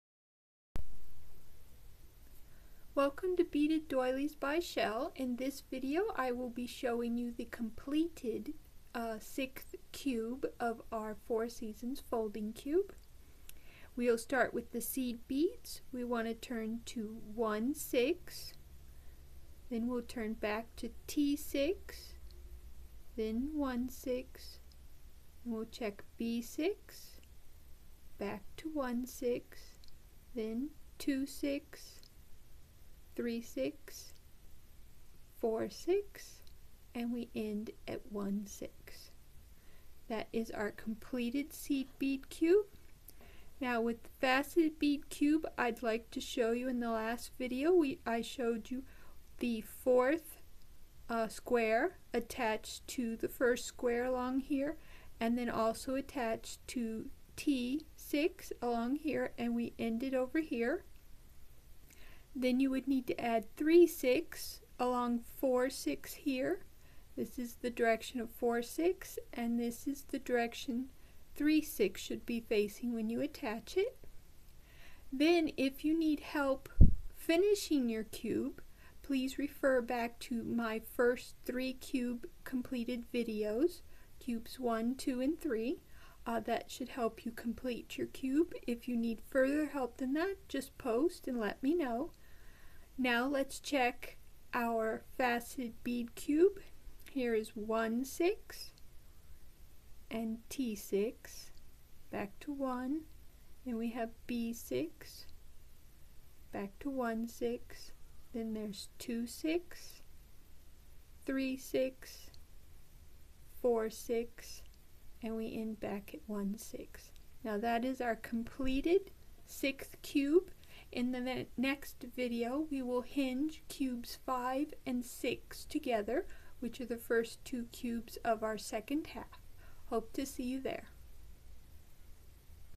VWC Four Seasons Folding Cube - the 6th cube
In this beading video I will be showing you the completed 6th cube of our Four Seasons Folding Cube.